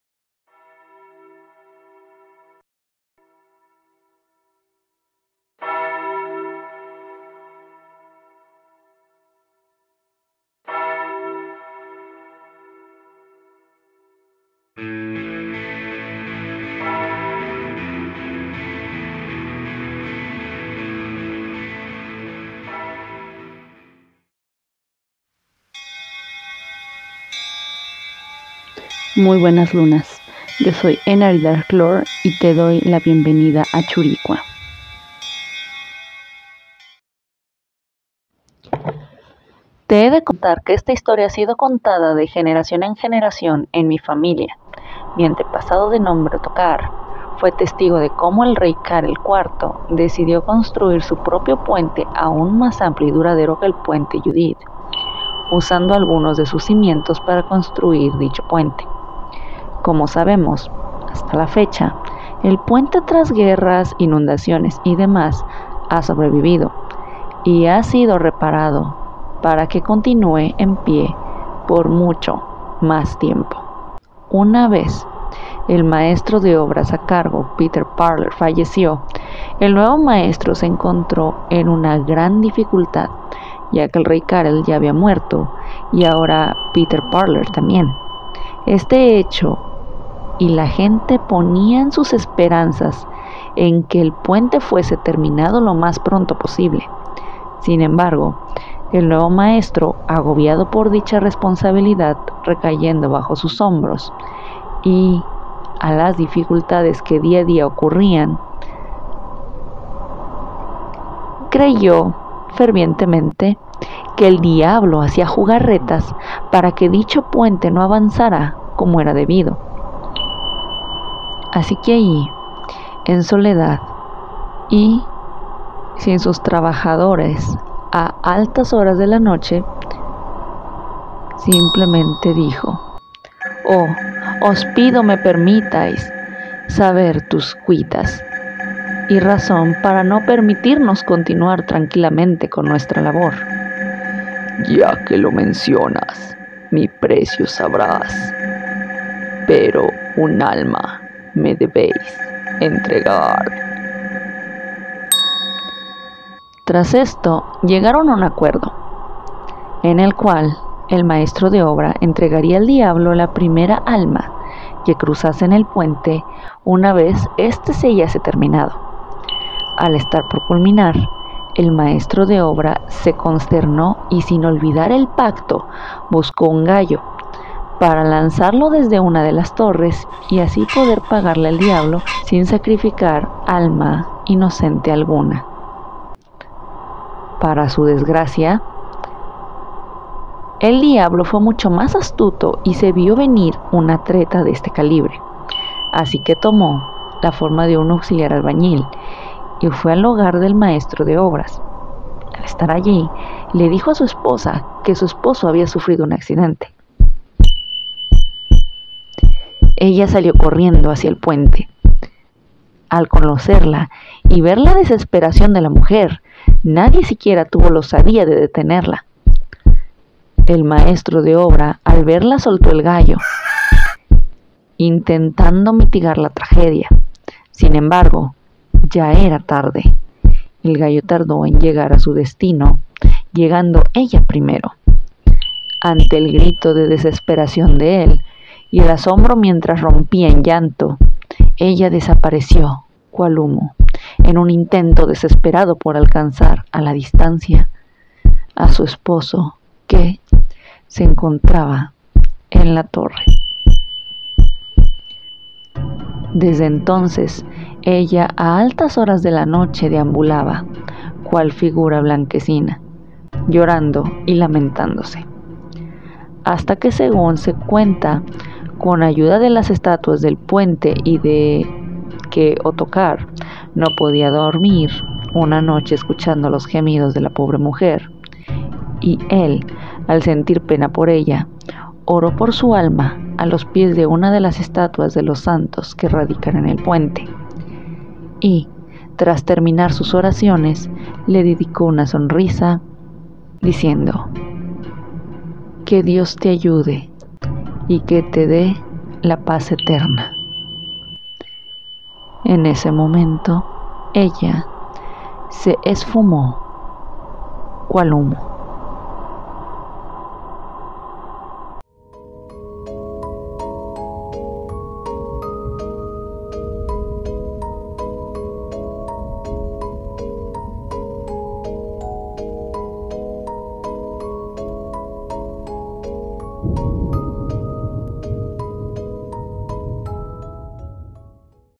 El diablo en Karlův Most
Cuando pagas con creces la solución a un problema, escucha esta historia de República Checa.